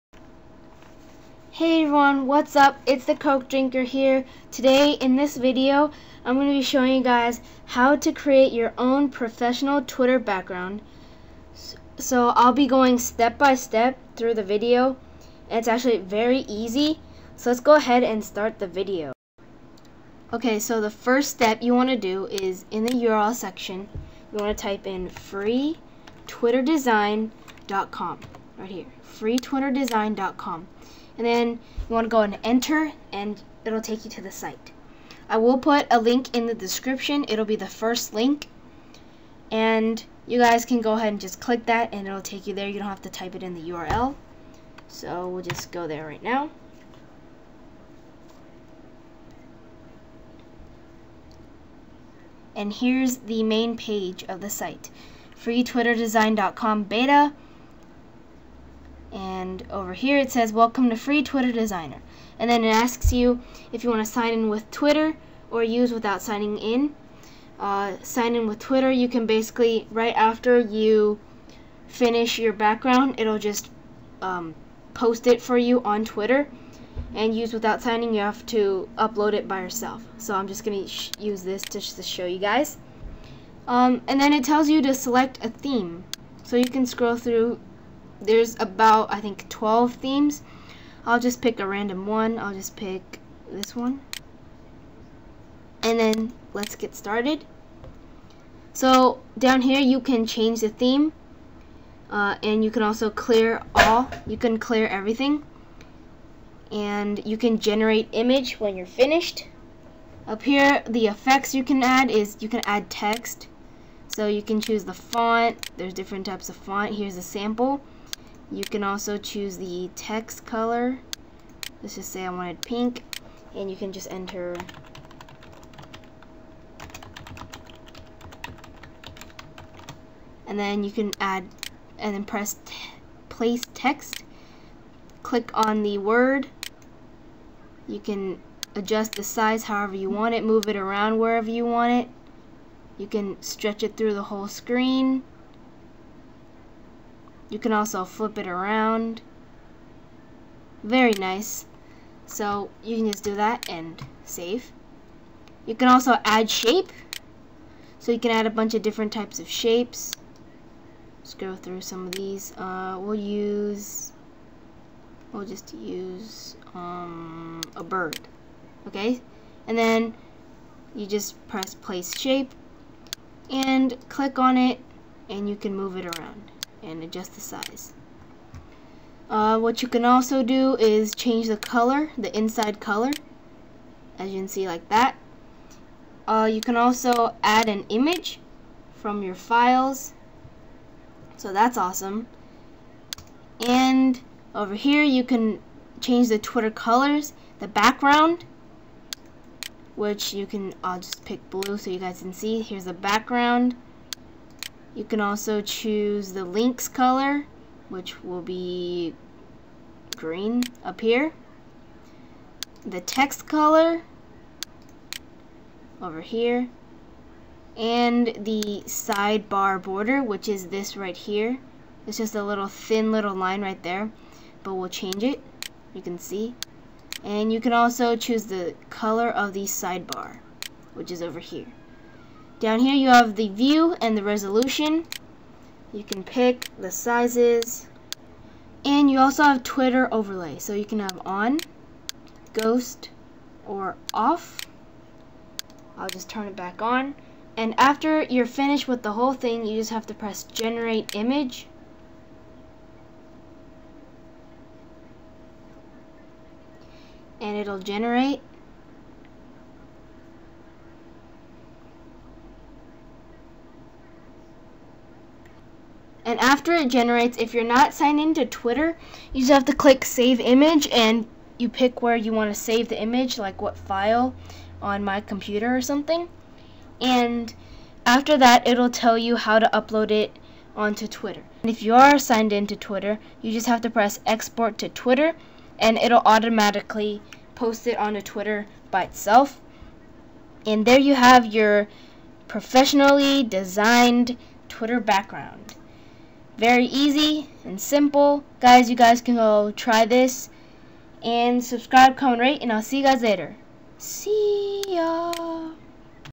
How to Create Your Own Twitter Background
this a professional way to make a twitter background its simple and easy give it a try!

my twitter to see the background i made!